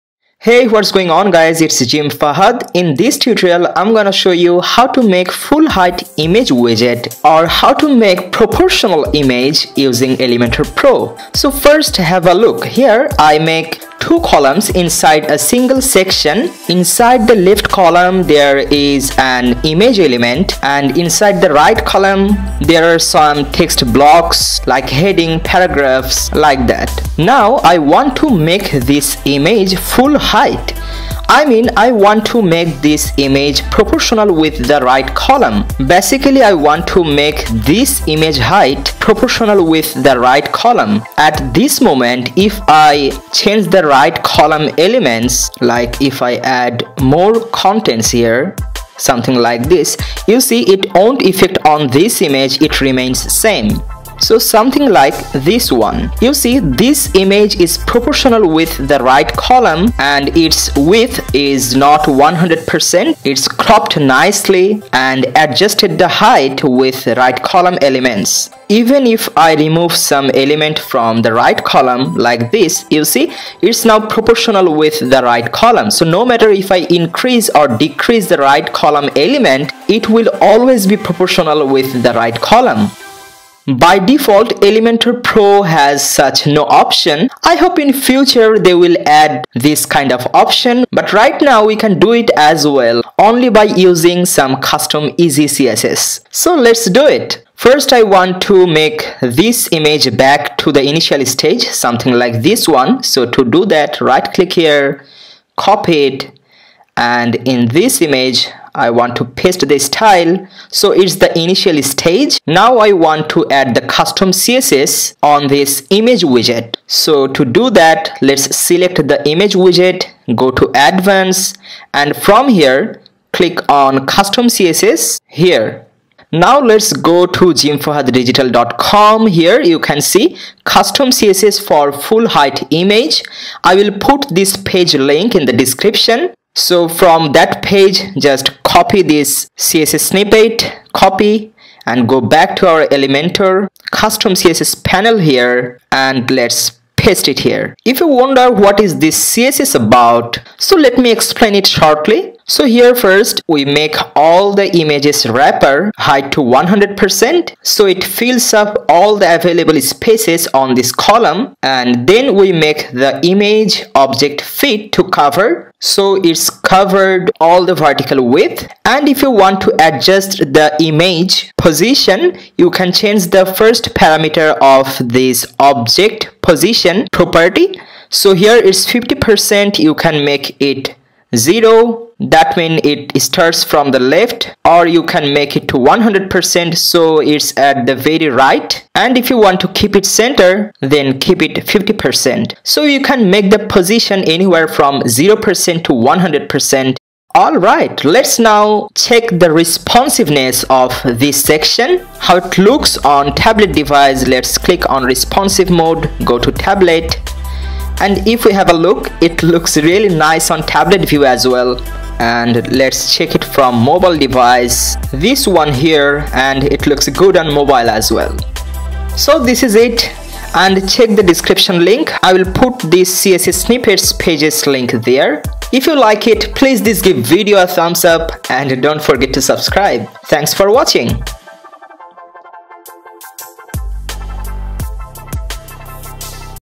How to Make Full-height/Proportional Image in Elementor PRO
In This Quick Tutorial, You will watch the easiest and simplest way How to Make Full-height Image in Elementor PRO. There's some easy custom CSS involved. I've shown it step-by-step. Let's enjoy How to Make Column proportional Image in Elementor PRO.

3 Million+ website owners started using the Elementor page builder. I'll show you the easiest way how you can create your own business website yourself.
Lots more tutorials are coming! I'll show you everything step-by-step.